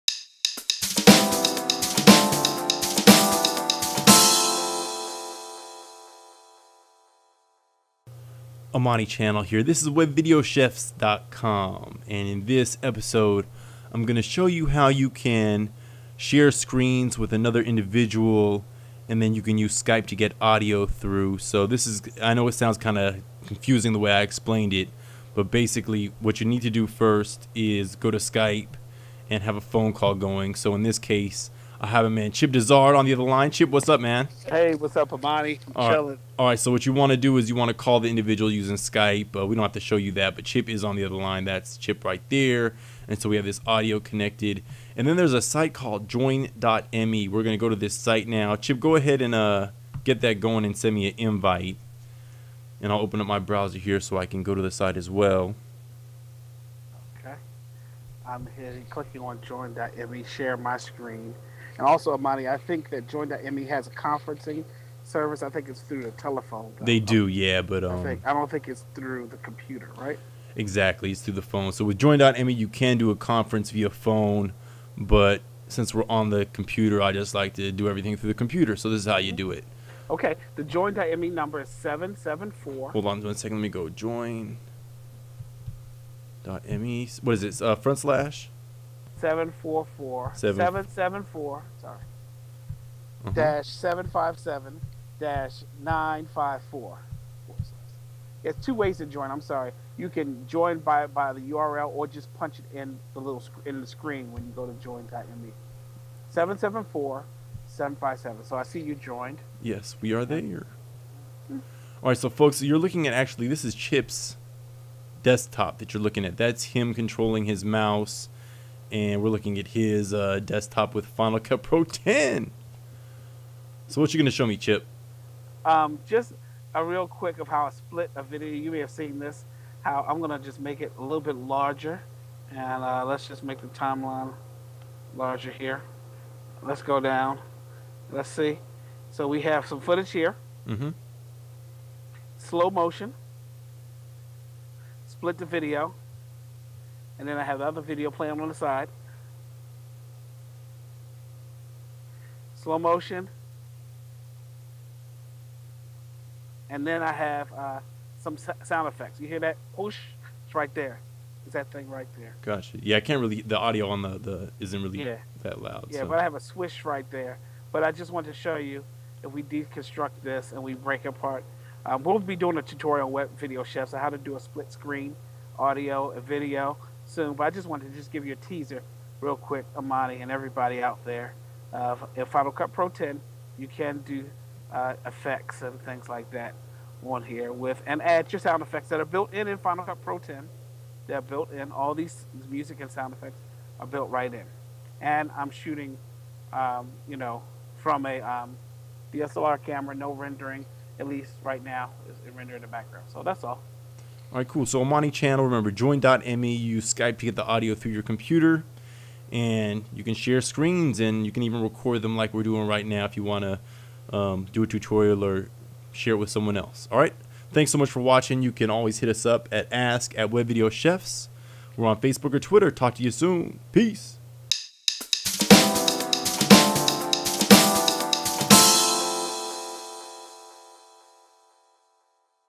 How to use Skype with Join.me
If you've ever wanted to screen share with someone, look no further than join.me. It's easy to use and free. The only issue is that you have to use a bridge line with the site.  If you're interested in getting audio through your computer, then watch this tutorial.